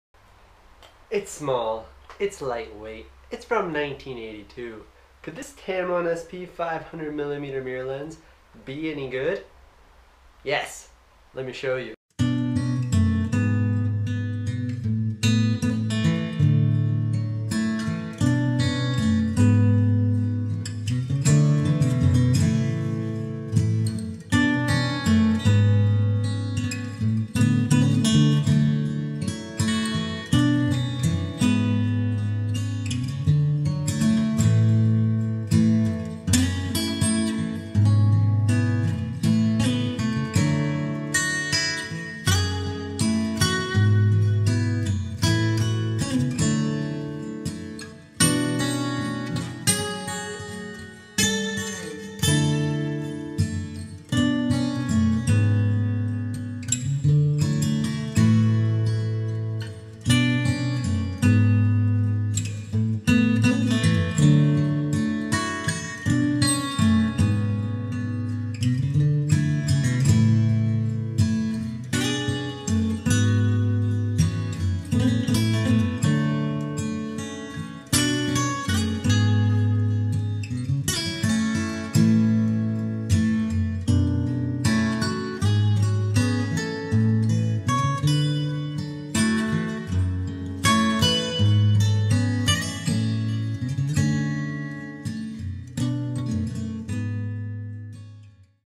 Tamron SP 500mm F8 Mirror Lens (55BB): Photo Samples
The Tamron SP 500mm is a classic mirror lens from 1982. It is lightweight, at around 600grams, it is small, at only 9cm, and it takes surprisingly sharp photographs! Don't believe me? Watch!